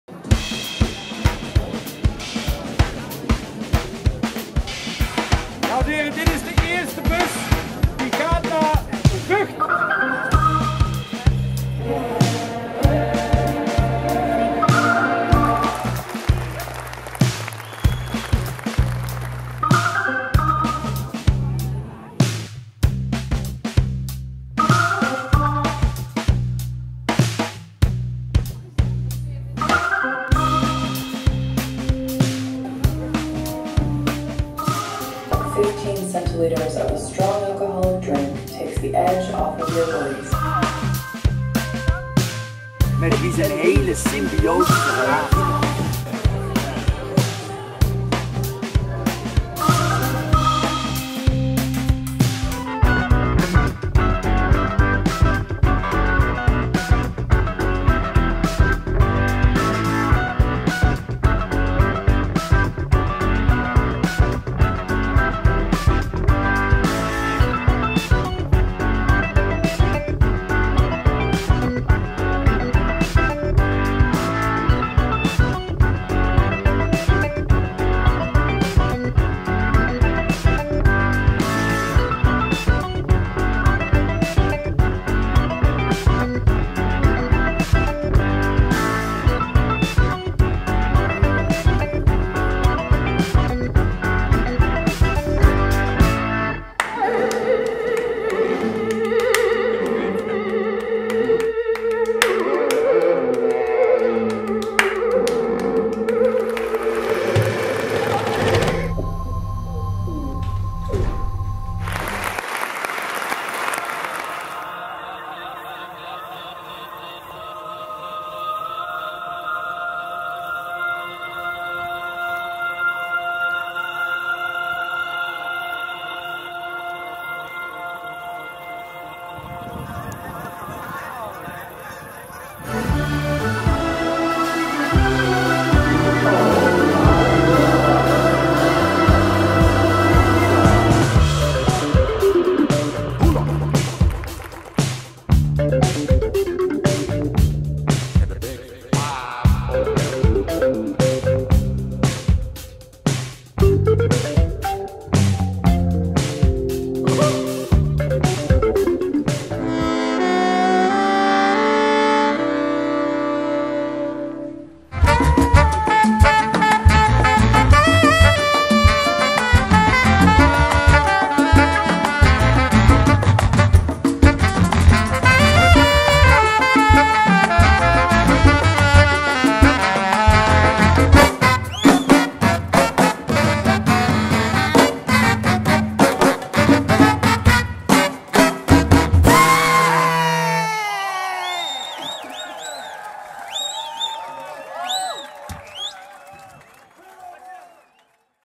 Theaterfestival Boulevard 2019: Aftermovie
De aftermovie van Theaterfestival Boulevard 2019, door Dave Menkehorst.